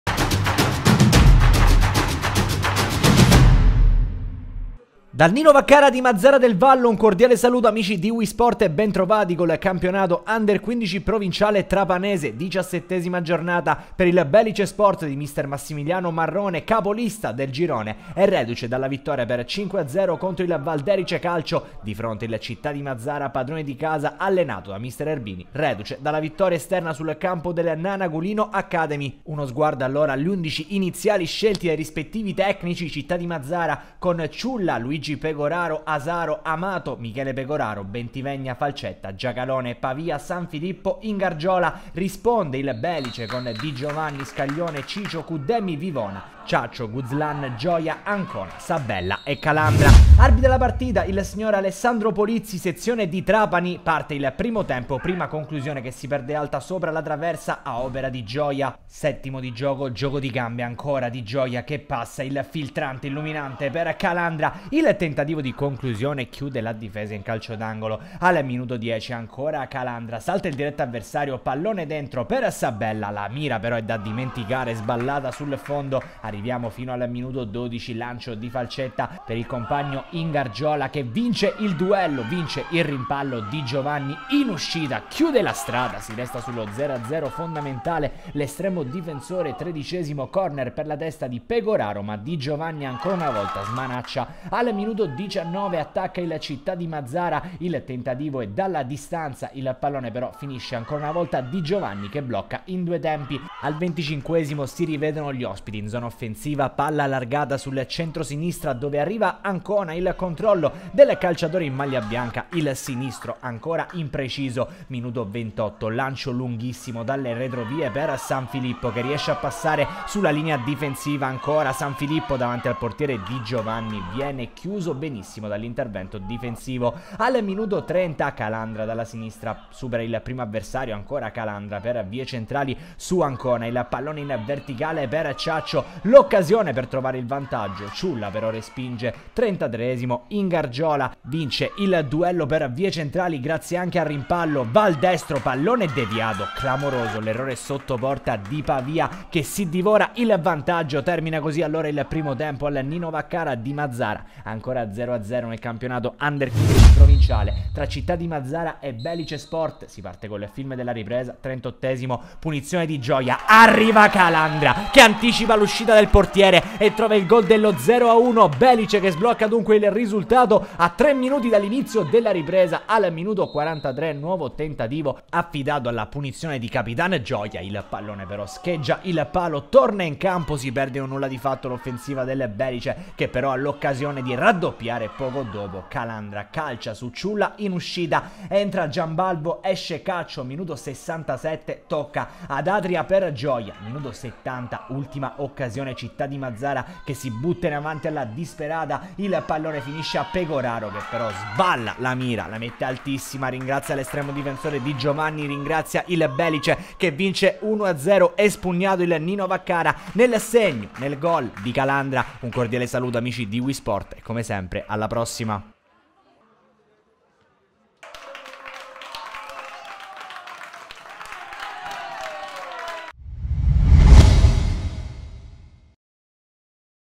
Città di Mazara vs Belice Sport (Under 15 Provinciale - 17G - Trapani)
Under 15 Provinciale - 17G - Trapani
Città di Mazara vs Belice Sport: 0-1

Marcatori: 38' Calandra

Servizio a cura di Francesco Morabito & Antonio Caroé